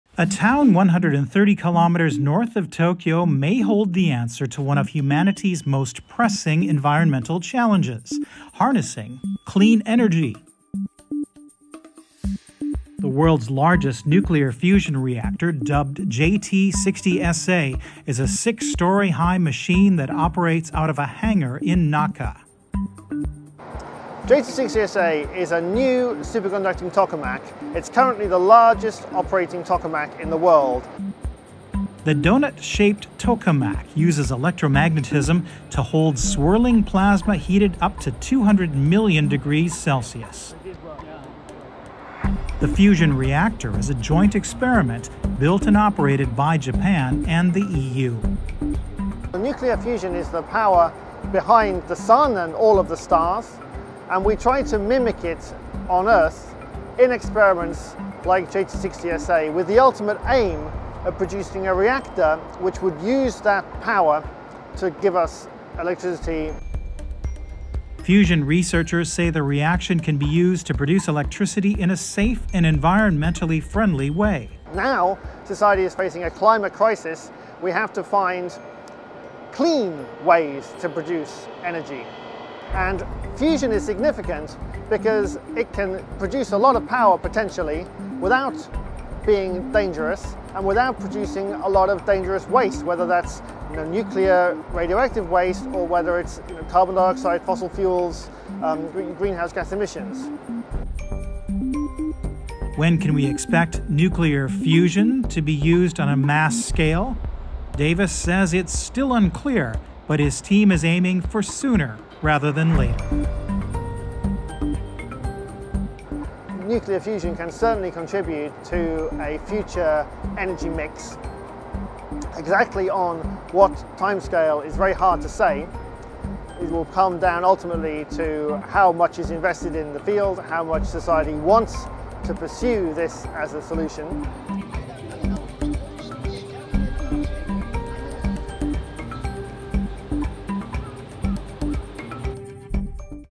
The world’s largest nuclear fusion reactor
The Japanese town of Naka may hold a solution to humanity’s climate crisis as well as its future energy needs. While the technology is still in its infancy, scientists at the world’s biggest experimental nuclear fusion reactor have been investigating the feasibility of such plants as a source of safer, large-scale and carbon-free energy.

Related story:
Chinese nuclear firm hails step forward in quest to build ‘artificial sun’